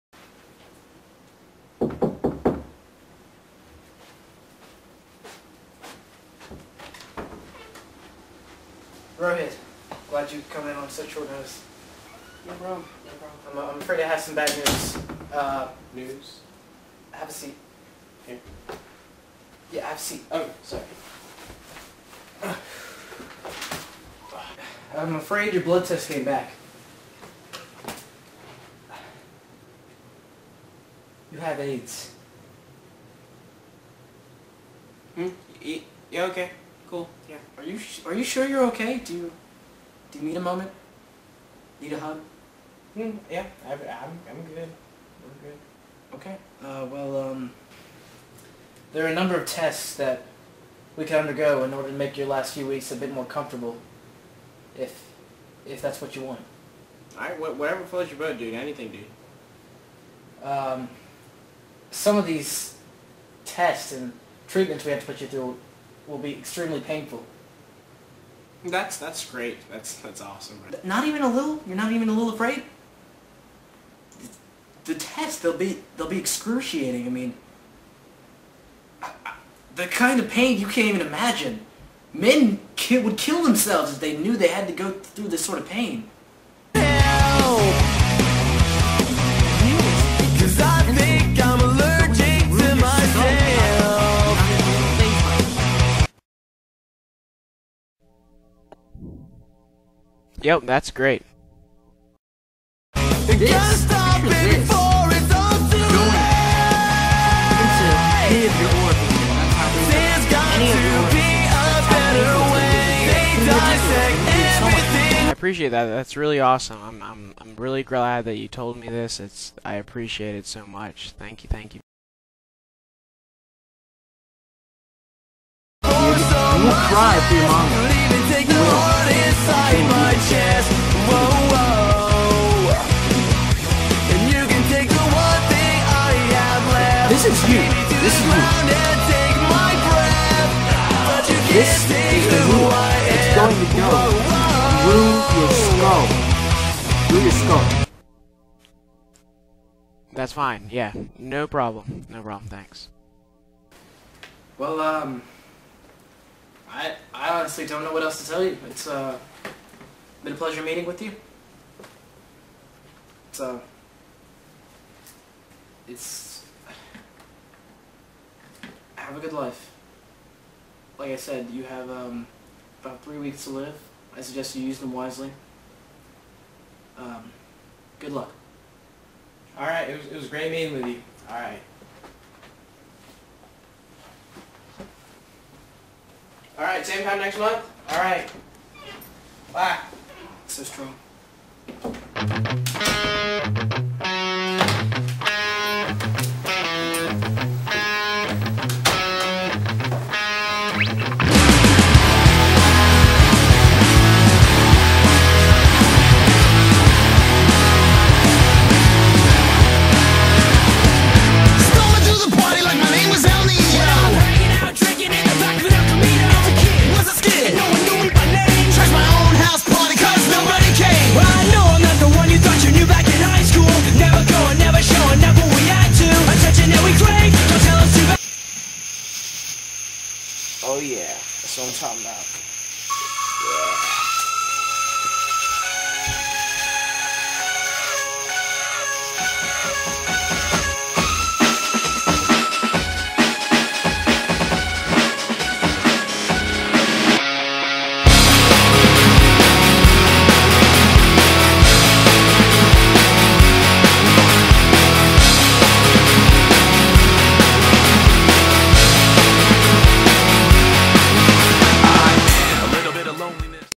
Bad News - Doctor
A young man is terrified to learn from his doctor that he has AIDS, or he would be if he could hear what he was saying.